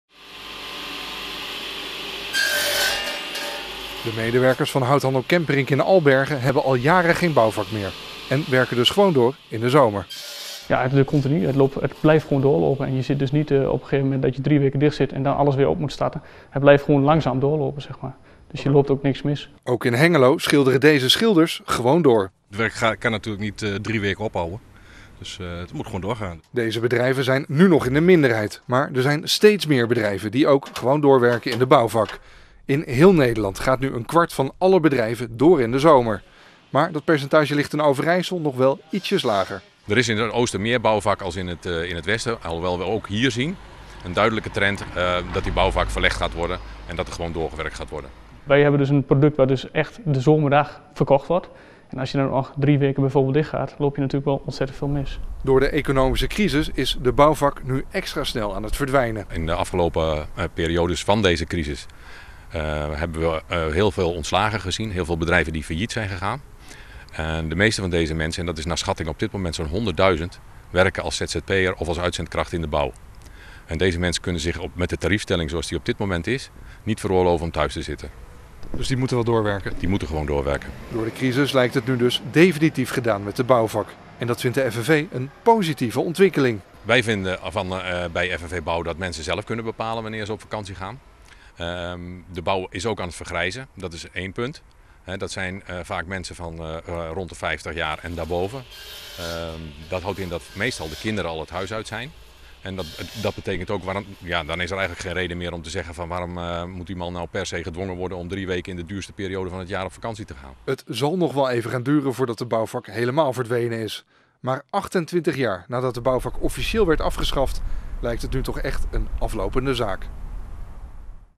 Kwart bouwbedrijven gaat door tijdens bouwvak
Een kwart van de bouwbedrijven werkt deze zomer gewoon door tijdens de traditionele bouwvak. Dat blijkt uit cijfers van Bouwend Nederland.

Traditioneel sluiten bouwbedrijven ieder jaar vier weken de deuren midden in de zomer. Ondermeer door de economische crisis gaan steeds meer bouwers gewoon door. Vakbond FNV is blij met die ontwikkeling.